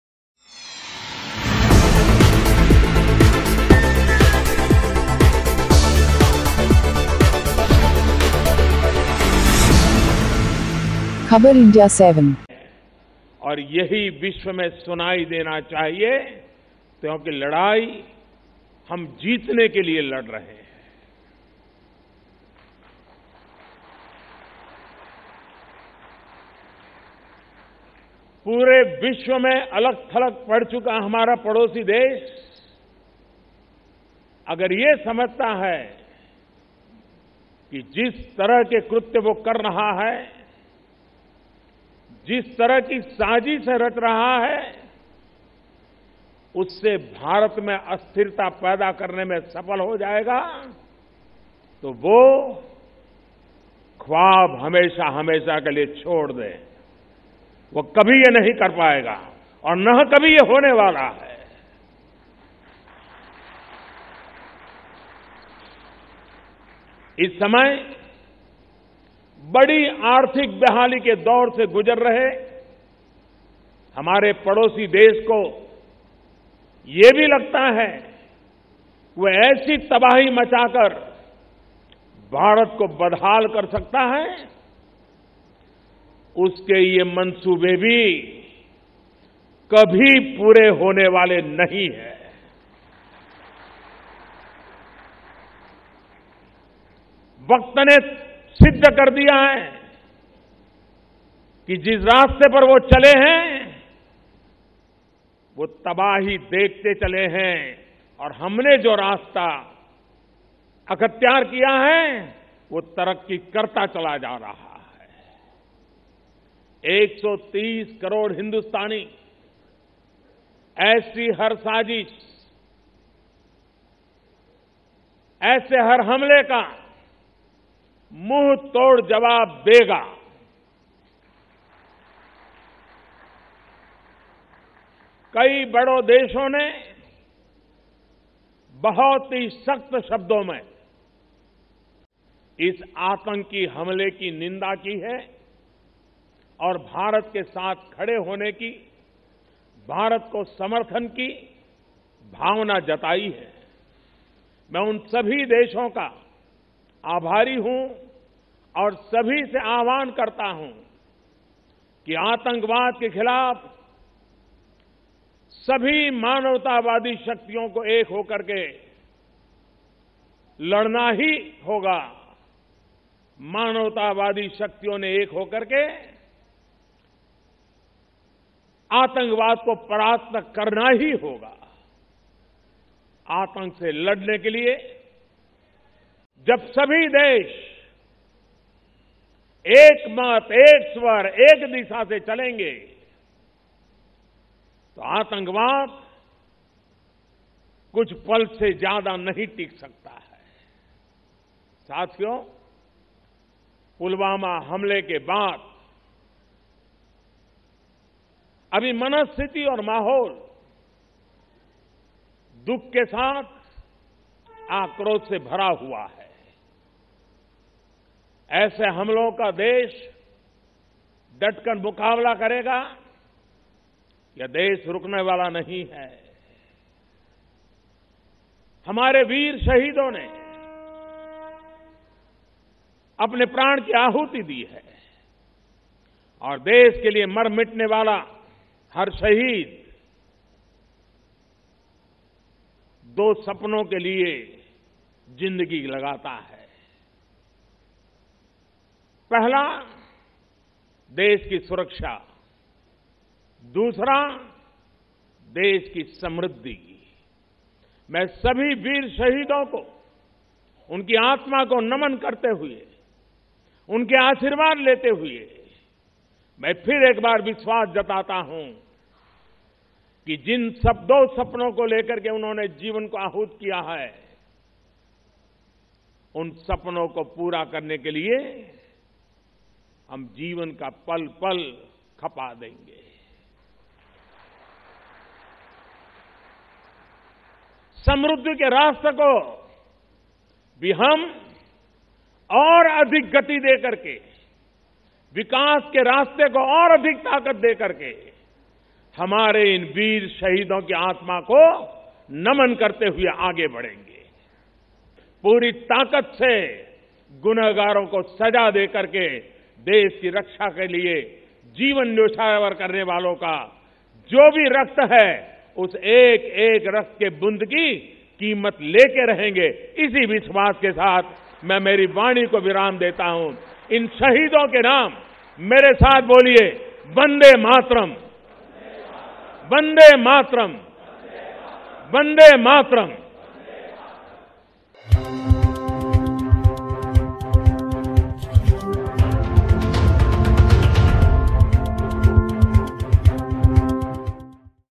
पुलवामा हमले को लेकर गुस्से में दिखे मोदी, पाक को दे डाली बड़ी चुनौती, पाक में मची खलबली !
Narendra Modi

पुलवामा हमले को लेकर गुस्से में दिखे मोदी, पाक को दे डाली बड़ी चुनौती, पाक में मची खलबली ! Watch Out Video !